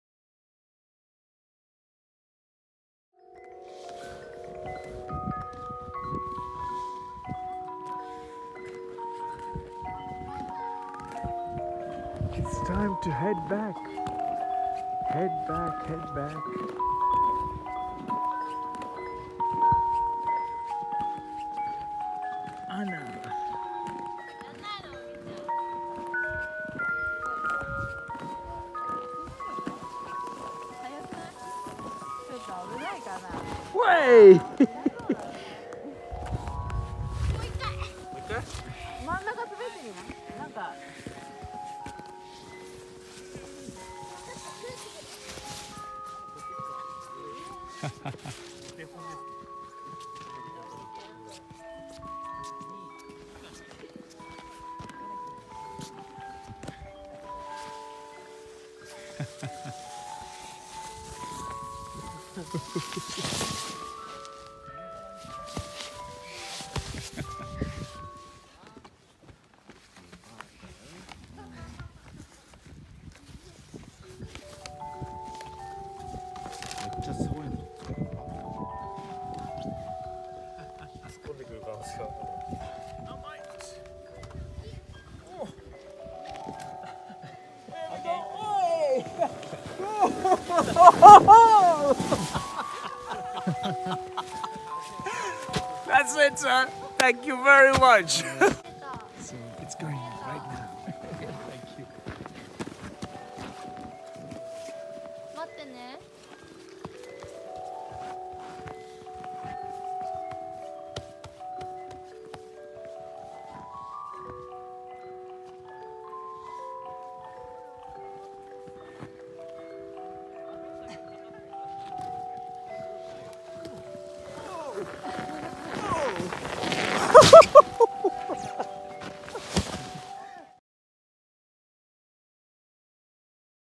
Sliding down Ice slides Lake Shikotsukoonsen
Sliding down Ice slides Lake Shikotsukoonsen, there were few snow sliding events at the snow festival in Hokkaido.